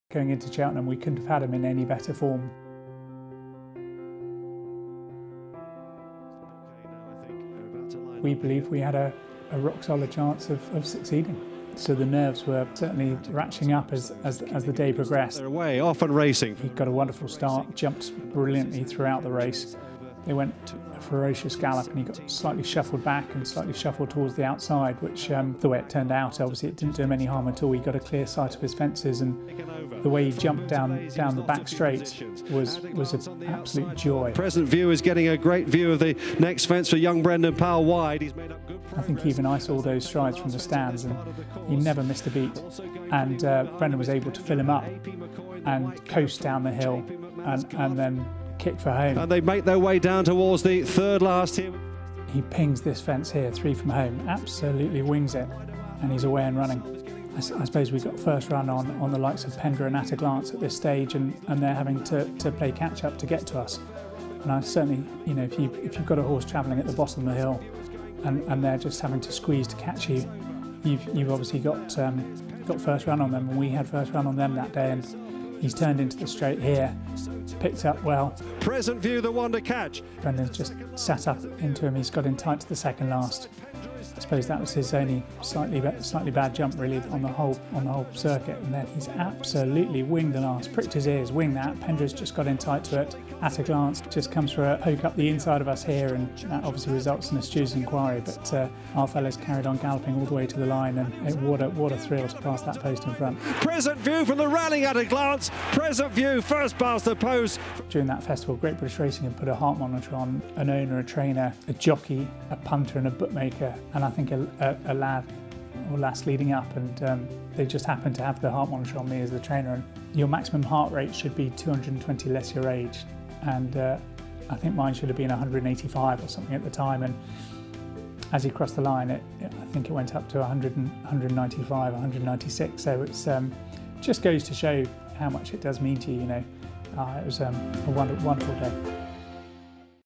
Present View wins at Cheltenham | Jamie Snowden on his first Cheltenham Festival winner
Trainer Jamie Snowden looks back on Present View's win in the 2014 Rewards4Racing Novices' Chase at the Cheltenham Festival.

The chaser was just Jamie's third ever runner at the Festival and owned by Arsenal Chairman Sir Chips Keswick.